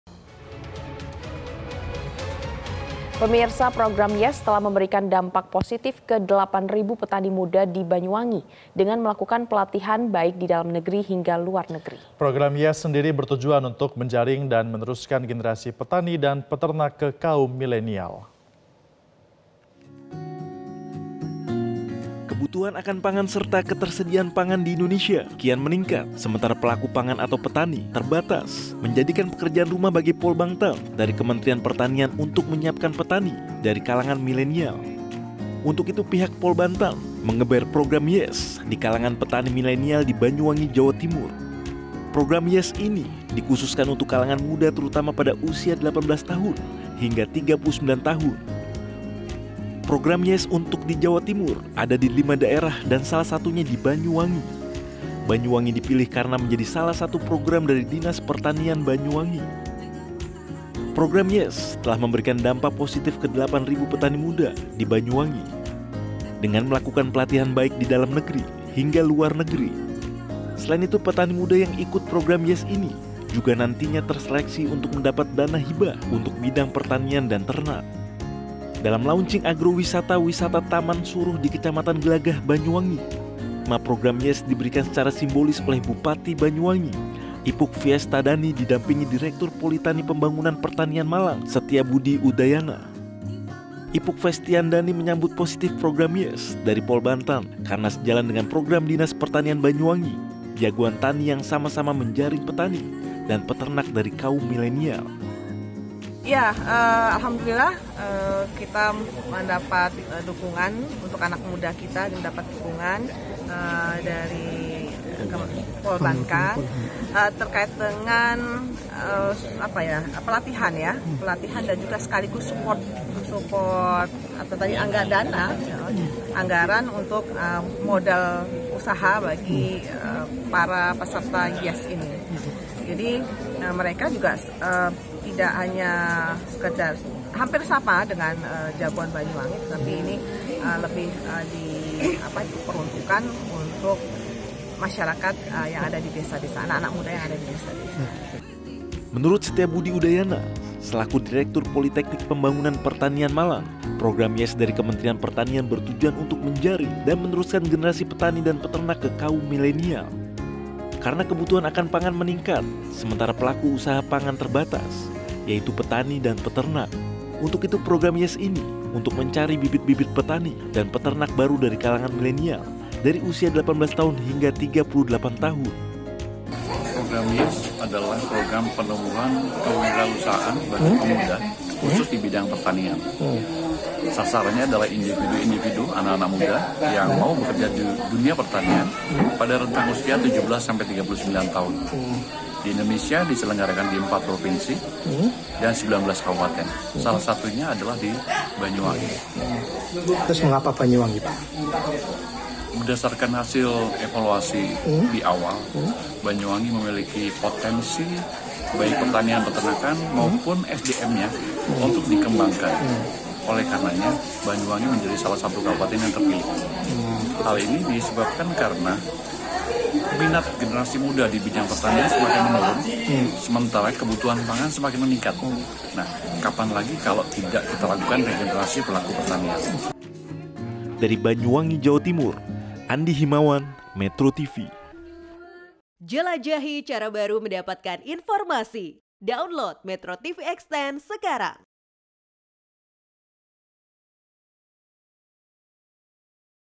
Polbangtan Malang Geber Program YESS di Banyuwangi
MetroTV, Program YESS telah memberikan dampak positif ke delapan ribu petani muda di Banyuwangi dengan melakukan pelatihan baik di dalam negeri hingga luar negeri. Program YESS sendiri bertujuan untuk menjaring dan meneruskan generasi petani dan peternak ke kaum milineal